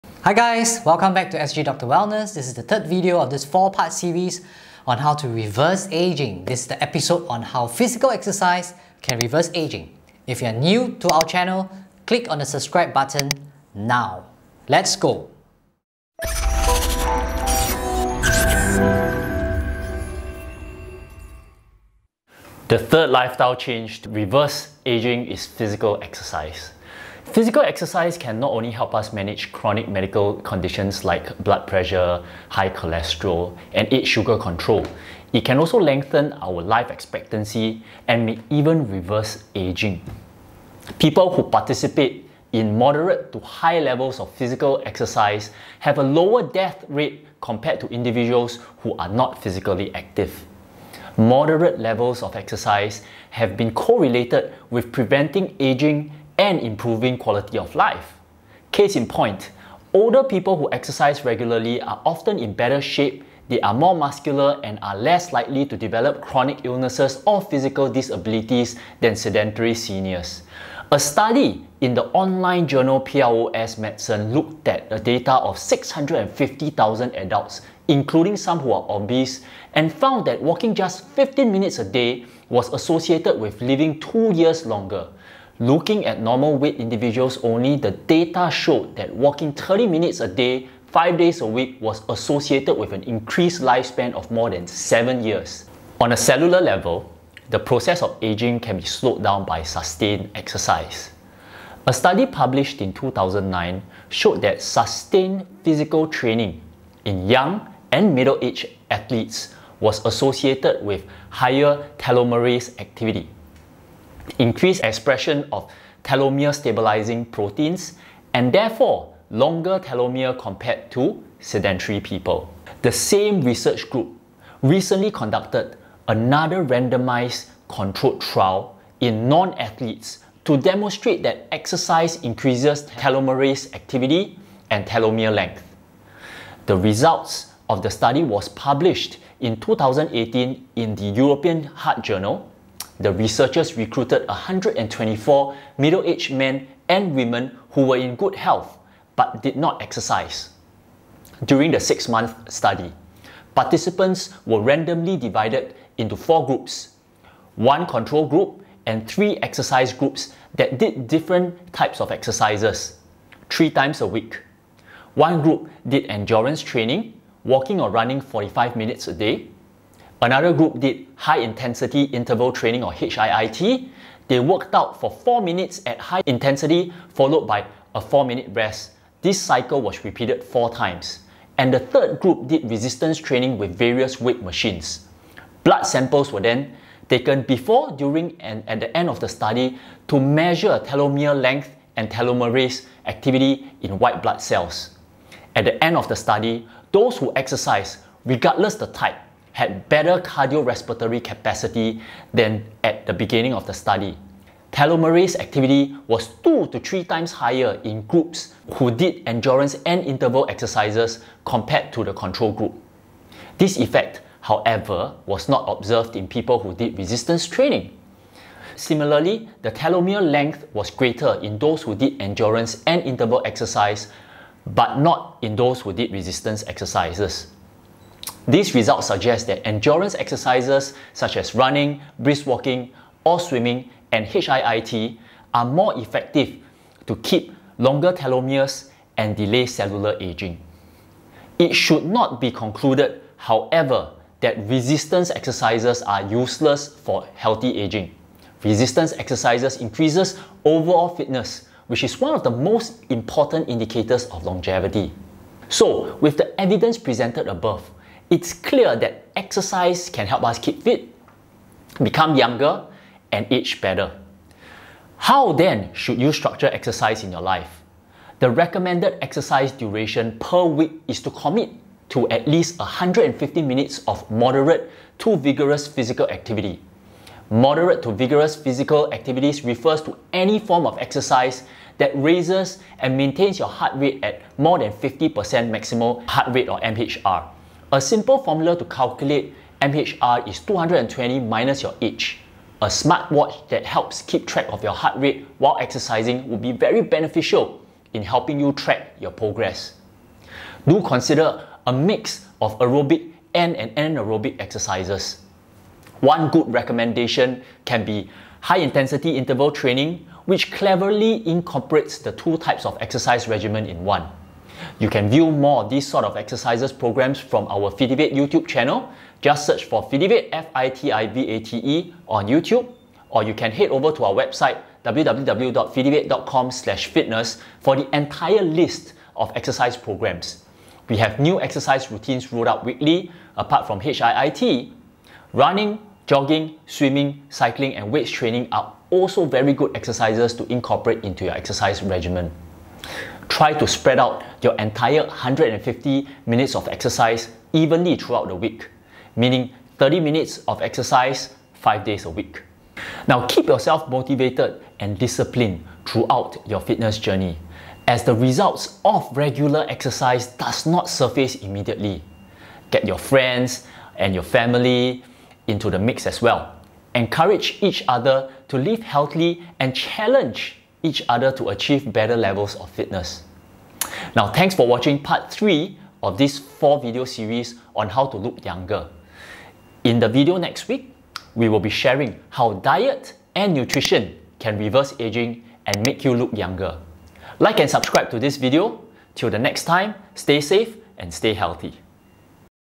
How to Reverse Aging Process (Part 3) - Physical Exercises
Hi guys, Welcome to the third video of this 4 part series on how to reverse ageing
This is the episode on how physical exercise can reverse ageing.

The third lifestyle change to reverse aging is physical exercise.
Physical exercise can not only help us manage chronic medical conditions like blood pressure, high cholesterol and aid with sugar control, it can also lengthen our life expectancy and may even reverse aging.
People who participate in moderate to high levels of physical exercise have a lower death rate compared to individuals who are not physically active.
Moderate levels of exercise have been correlated with preventing ageing and improving quality of life.
Case in point, older people who exercise regularly are often in better shape, they are more muscular, and they are less likely to develop chronic illnesses or physical disabilities than sedentary seniors.

A study in the online journal PLoS Medicine looked at data on 650,000 adults ( including some who were obese ) and found that walking just 15 minutes a day was associated with living 2 years longer.
Looking at normal-weight individuals only, the data showed that walking 30 minutes a day, 5 days a week was associated with an increase in a life span of more than 7 years.

On a cellular level, the process of ageing can be slowed down by sustained exercise.
A study published in 2009 showed that sustained physical training in young and middle-aged athletes was associated with higher telomerase activity, increased expression of telomere-stabilising proteins and therefore longer telomeres, compared to sedentary people.
The same research group recently conducted another randomised controlled trial in non-athletes to demonstrate that exercise increases telomerase activity and telomere length.
The results of the study were published in 2018 in the European Heart Journal.
The researchers recruited 124 middle-aged men and women who were in good health but did not exercise.
During the 6 months study, participants were randomly divided into four groups.
1 Control group and three exercise groups that did different types of exercises 3 times a week.
1 group did endurance training ( walking or running 45 min a day )
Another group did high-intensity interval training. They worked out for 4 min at high intensity followed by a 4 min rest - this cycle was repeated 4 times.
And the third group did resistance training with various weight machines.
Blood samples were taken before, during, and at the end of the study to measure telomere length and telomerase activity in white blood cells.
At the end of the study, those who exercised, regardless of the type, had better cardiorespiratory capacity than at the beginning of the study.
Telomerase activity was 2-3 times higher in the group who did endurance or interval exercises, compared to the control group.
This effect, however, was not observed in people who did resistance training.
Similarly, the telomere length was greater in those who did endurance or interval exercises, but not in those who did resistance exercise.
These results suggest that endurance exercises such as running, brisk walking, or swimming and high-intensity interval training are more effective to keep longer telomeres and delaying cellular ageing.
The recommended exercise duration per week is to commit to at least 150 mins of moderate to vigorous physical activity.
Moderate to vigorous physical activities refers to any form of exercise that raises and maintains your heart rate at more than 50% maximal heart rate.

Do consider a mixture of aerobic and anaerobic (resistance training) exercises.
One good recommendation can be high intensity interval training which cleverly incorporates the 2 types of exercise regimen in one.
You can view more of these sort of workout programs from our Fitivate youtube channel, just search for FITIVATE on youtube or you can head over to our for the entire list of exercise programs.
Try to spread out your entire 150 mins of exercise evenly thoughout the week - meaning 30 minutes of exercise 5 days a week.

0:00 intro
0:22 reverse aging through physical exercise
1:35 how exercise reduce cellular aging
4:15 how to structure exercise into your life
5:14 keep fit with fitivate.com